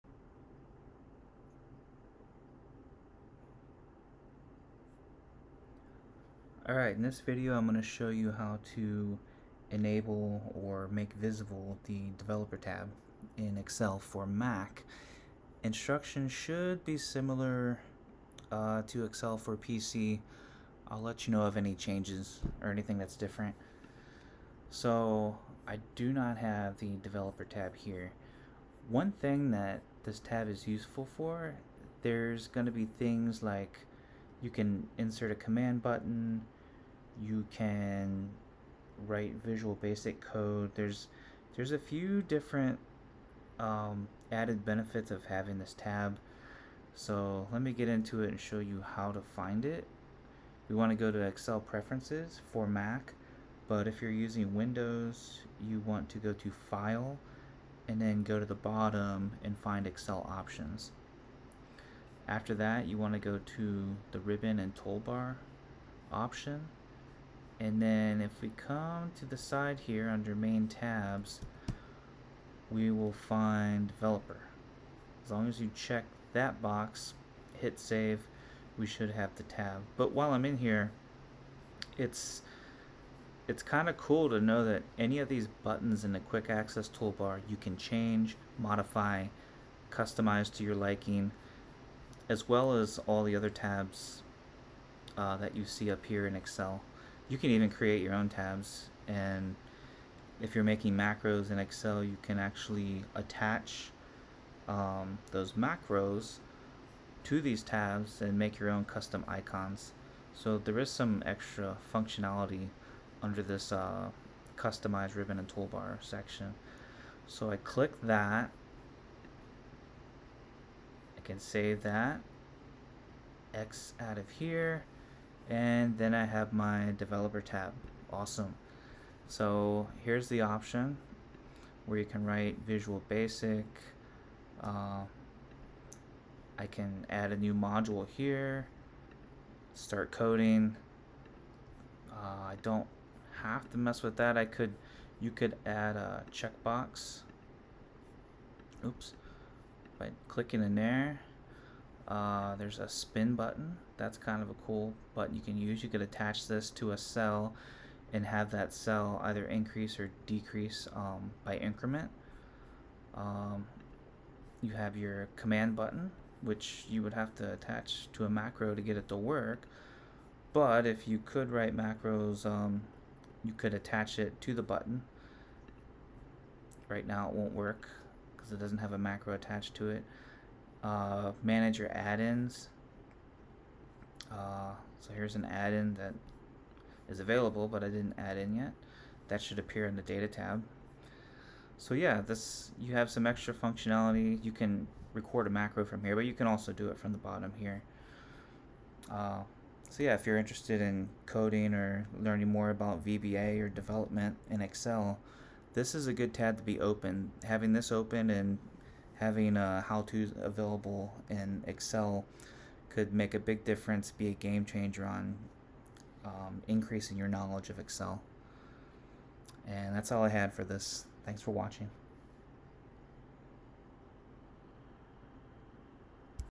Add The Developer Tab To The Excel Ribbon - Excel For Mac
What makes using Excel fun for me is the fact that I'm still learning how to use it in different ways. Having the developer tab available in Excel can open the door to learning. There is so much you can do with VBA code as a developer in Excel.

By default, the developer tab is not displayed on the Excel ribbon. To use the developer tab, it must be added by customizing the ribbon.

CHAPTERS
00:00 Start
00:32 Usefulness of Developer Tab
00:48 Select Preferences
01:13 Developer CheckBox
02:08 Explore Developer Tab
02:11 Add A Module